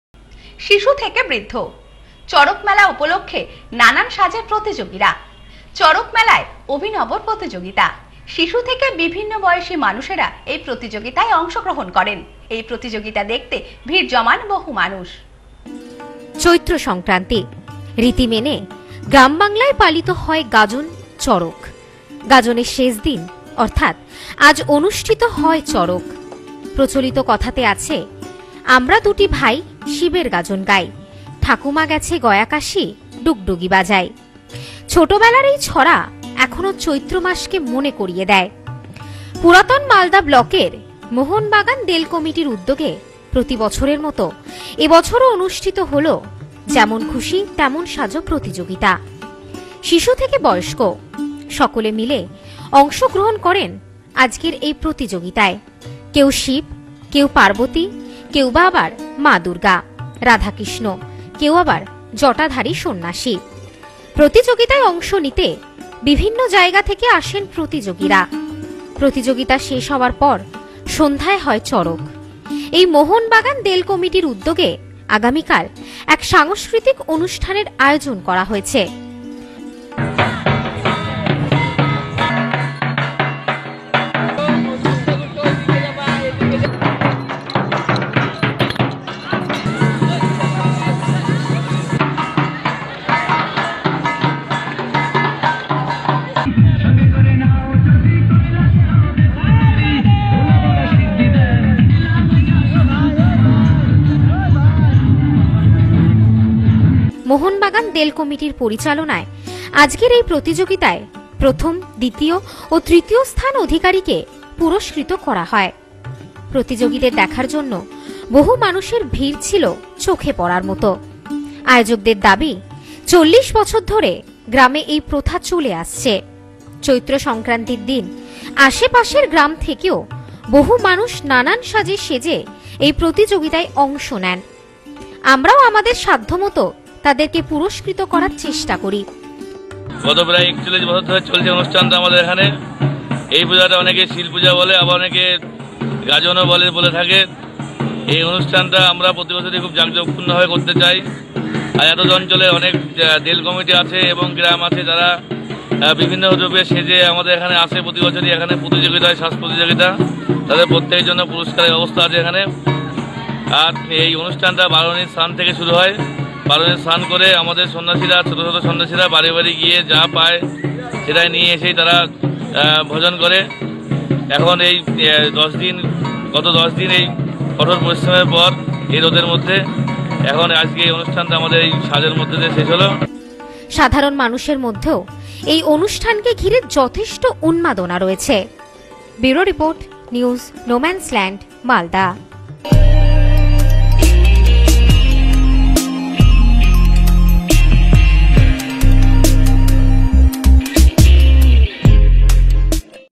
শিশু থেকে বৃদ্ধ চড়কের মেলায় নানান সাজে প্রতিযোগিরা।
শিশু থেকে বৃদ্ধ চড়কের মেলায় নানান সাজে প্রতিযোগিরা। চড়ক উপলক্ষে অভিনব প্রতিযোগিতা।শিশু থেকে বিভিন্ন বয়সী মানুষ অংশ নিলেন প্রতিযোগিতায়। বহু মানুষ ভীড় জমান এদিনের অনুষ্ঠানে।